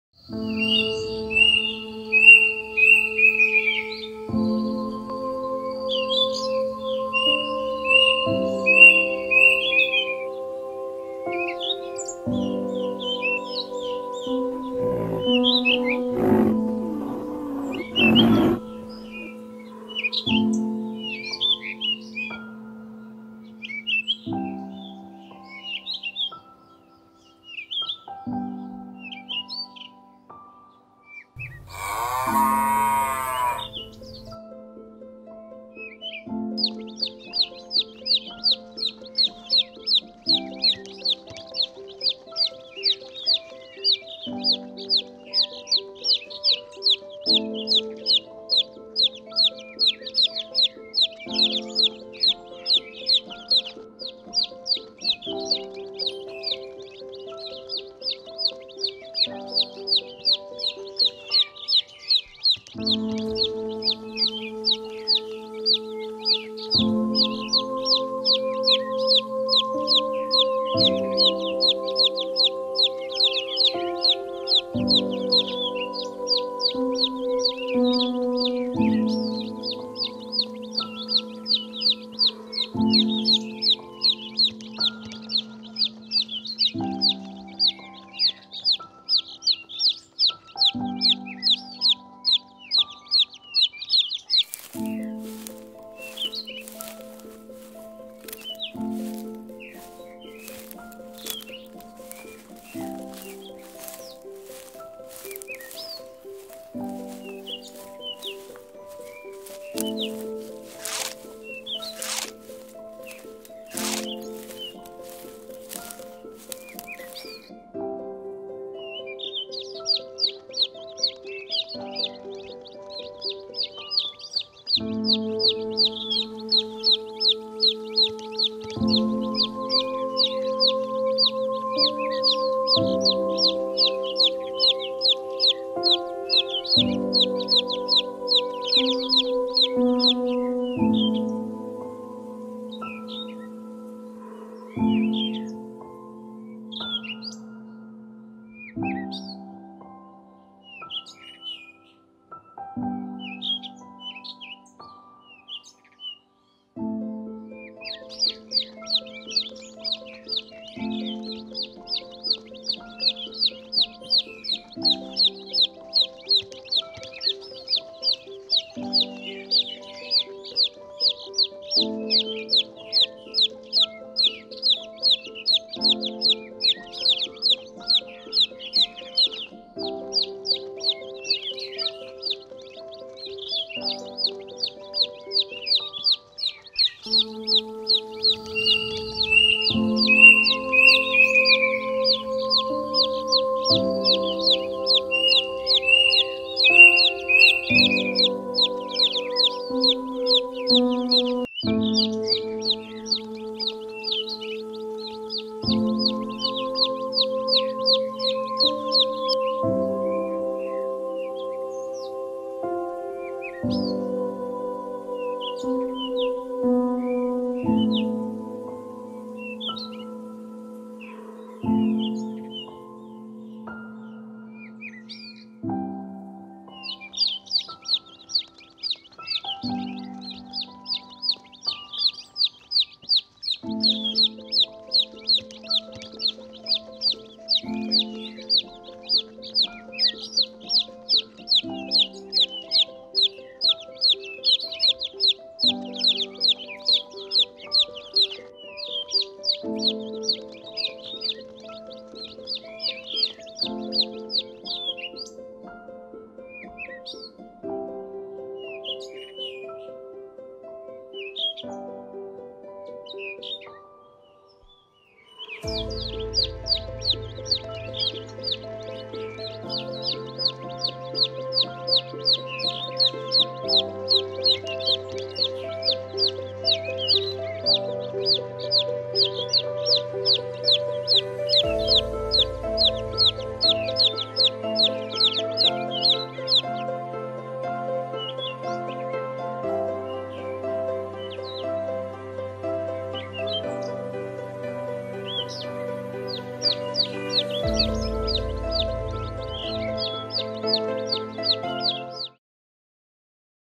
house of animals | Colour Chicks Lazy Wakeup | chicks playing | DOC JOPER
this video contains only for entertainment purpose , not for animal abuse, we are take care about chicks with the help of veterinary doctor and providing heat generation for survival of docjoper chicks...